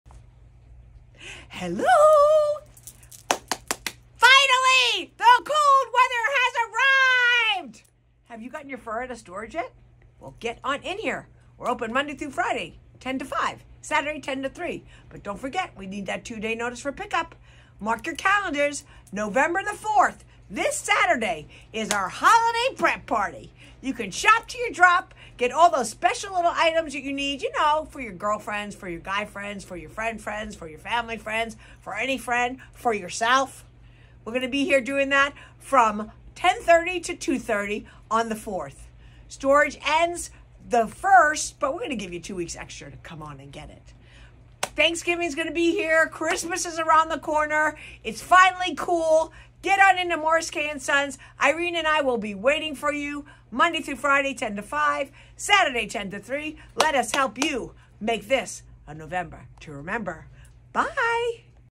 Carol's Corner November 2023
Embrace the charm of November with Morris Kaye & Sons! In this month's edition of Carol's Corner, Carol brings in the season's greetings, invites you to our Holiday Prep Party on November 4th (10:30 a.m. - 2:30 p.m.), and encourages you to get a jump start on your holiday shopping. Expect an atmosphere filled with festive joy, delightful company, and the very best of our collection!

Ready for fur pick-up? We’ve got everything in place to assist you! Stop by, catch up, and let's make your November special.

Can't wait to see you at the party and in-store! 🎉🧣